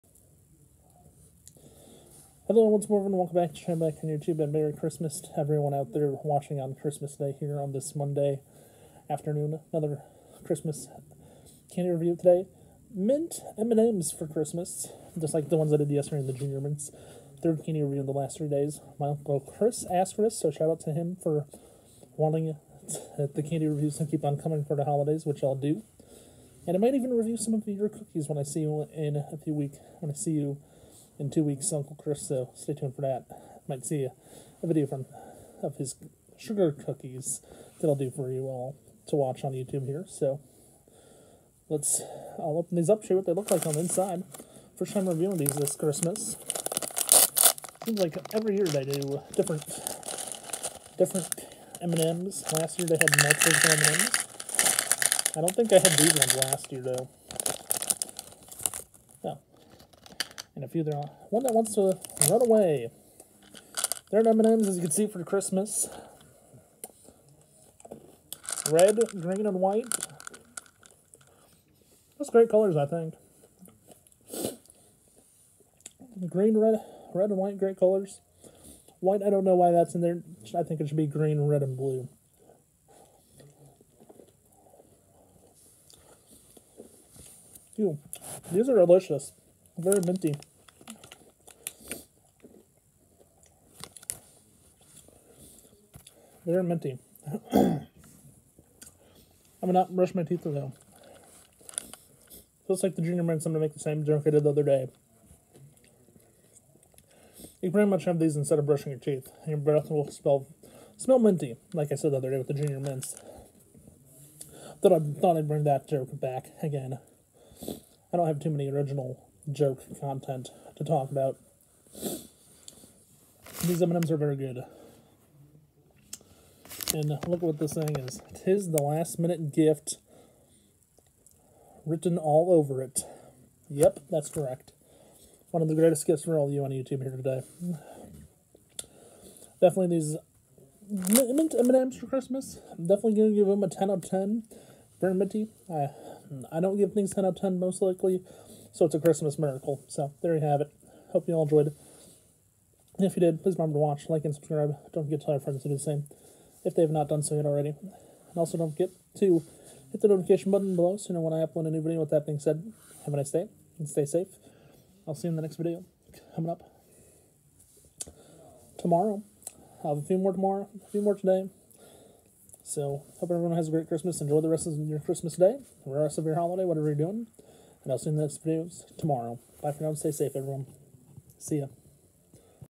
M&M’s Mint Review
Here is the newest candy review of the M&M’s Mint version which is out for the holidays. These M&M’s are very sweet and tasty and that is why I am going to give them a 10 out of 10. I hope everyone enjoys the video.